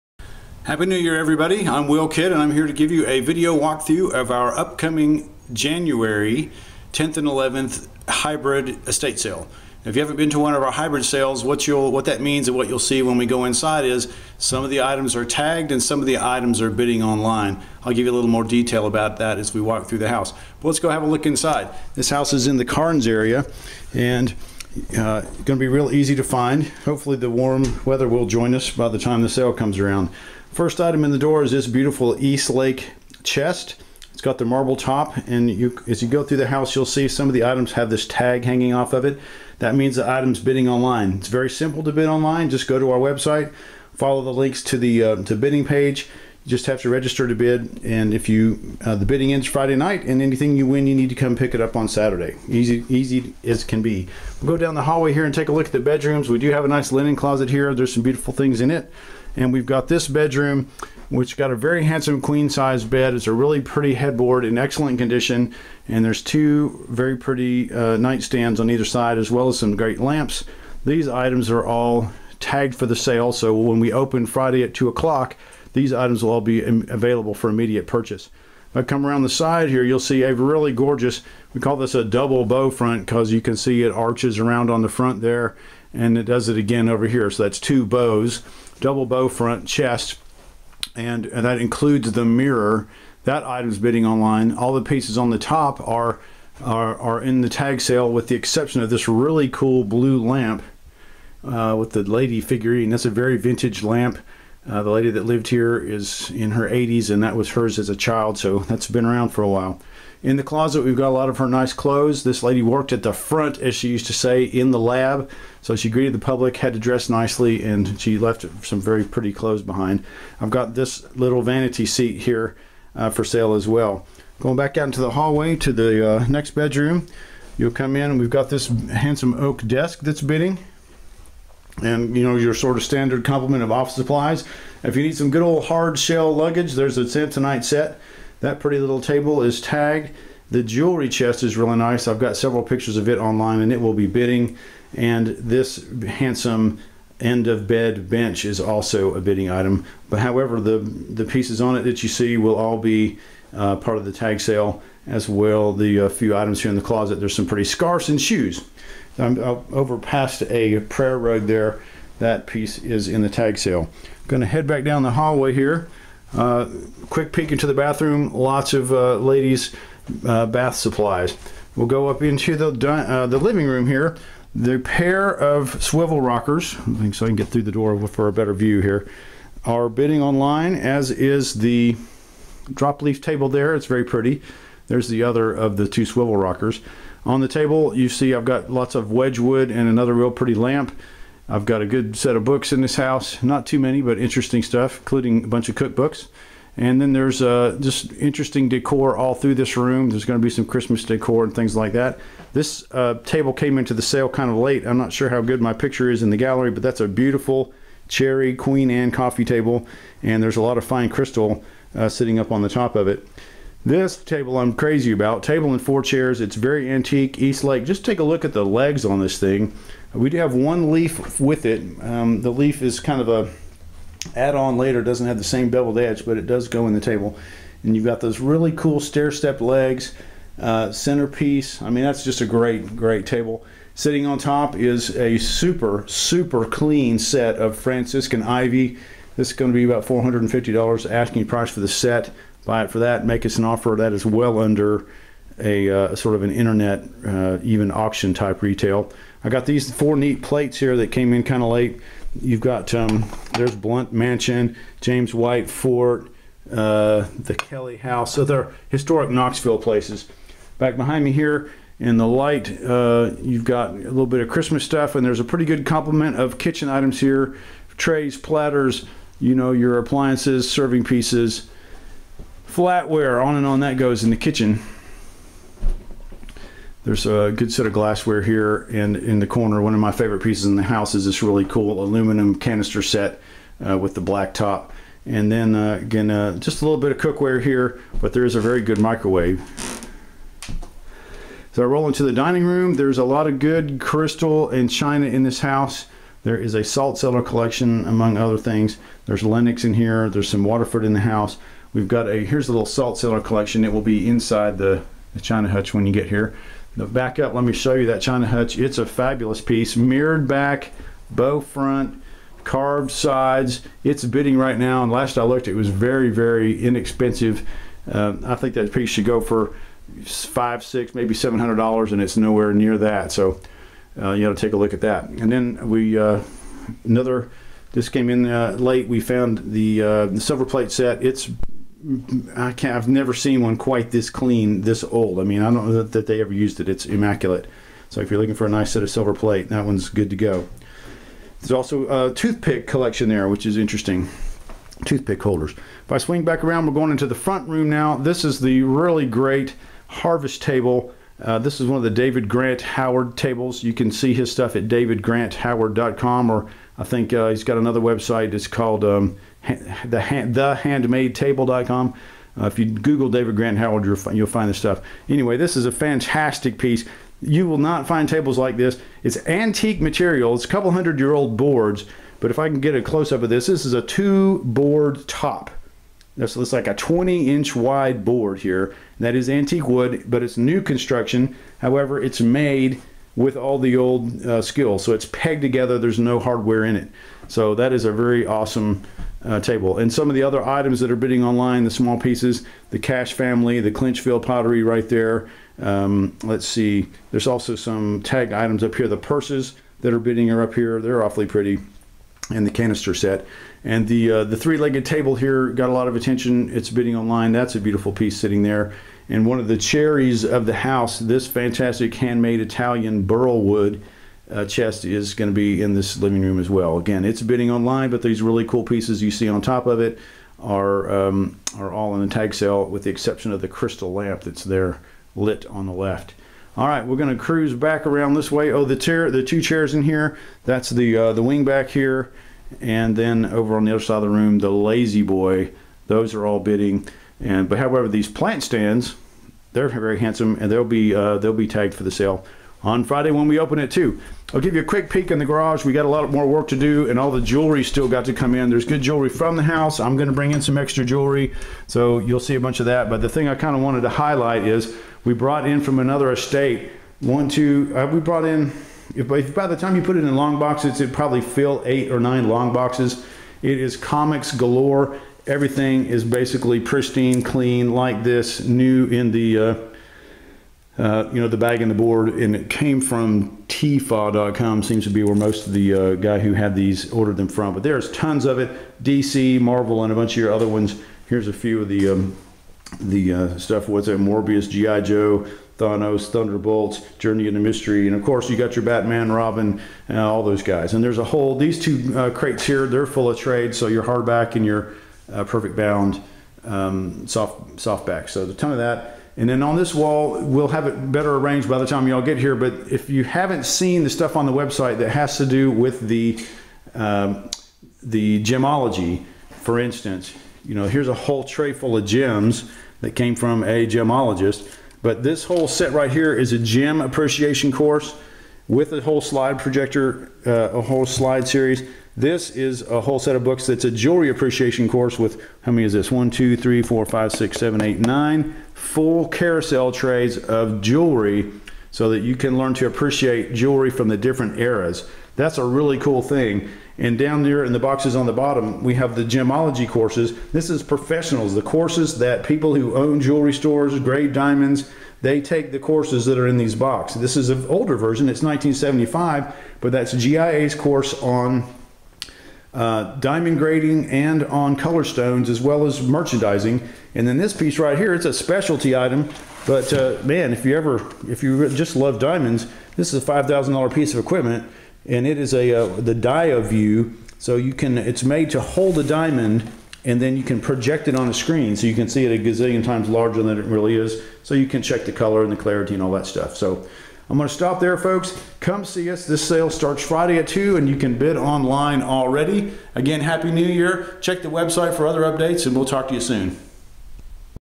Jan 10-11 Hybrid Estate Sale Walk-through
This video shows the contents of a home that will be sold via Hybrid Estate Sale on the weekend of Jan 10-11, 2014 in the Karns area of Knoxville, TN.  Portions of these items will be sold via Online Bidding and the balance by Public Tag Sale in the home.